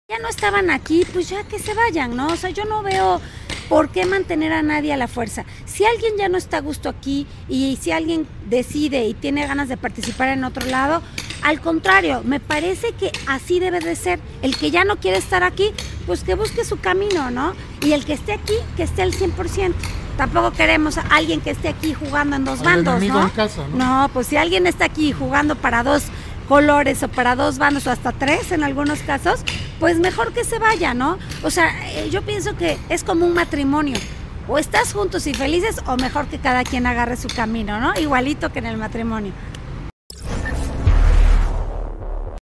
El que ya no quiera estar en el PAN que se vaya: Mónica Rodríguez
🧿El que ya no quiera estar en el PAN que se vaya, ya que los partidos son como el matrimonio, sentenció Mónica Rodríguez Della Vecchia ante la renuncia de GuillermoVelázquez

📹Alejandro Camacho